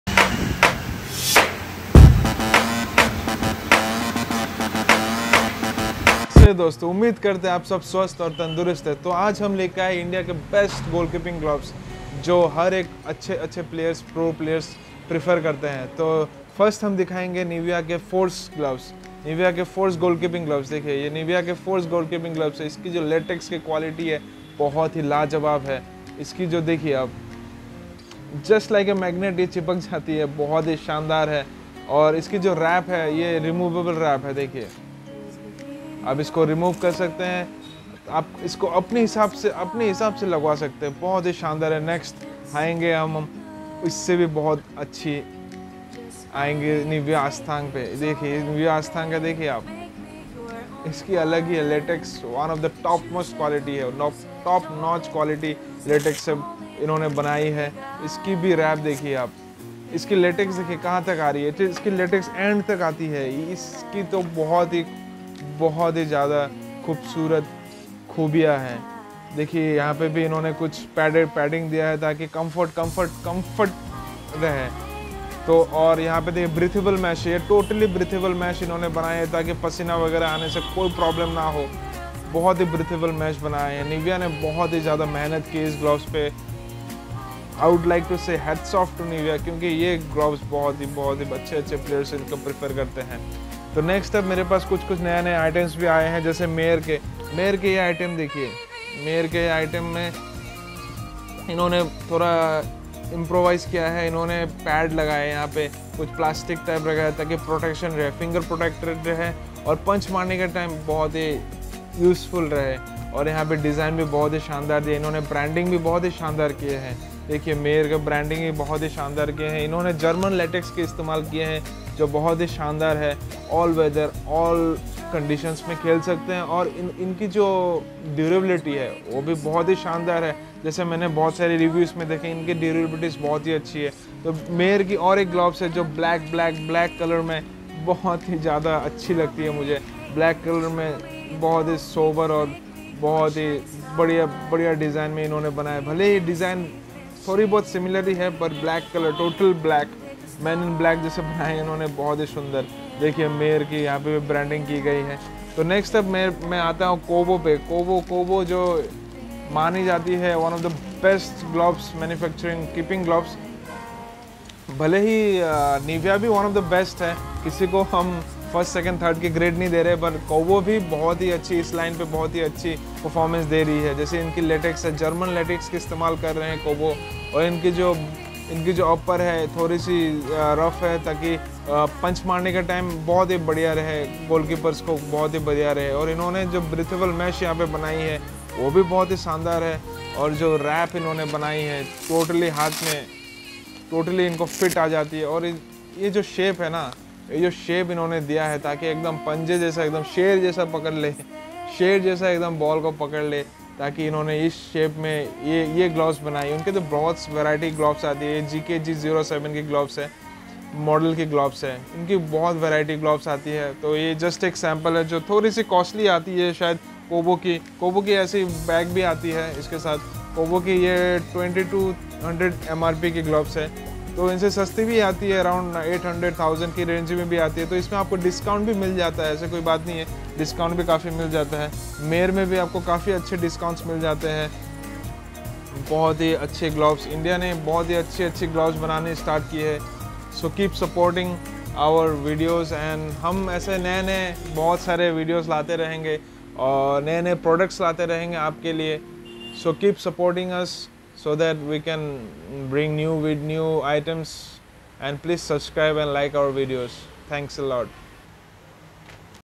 Best Goalkeeping Gloves for all the football players
Best goalkeeping gloves from nivia , mayor and kobo at an affordable and budget friendly price for all the senior goalkeeper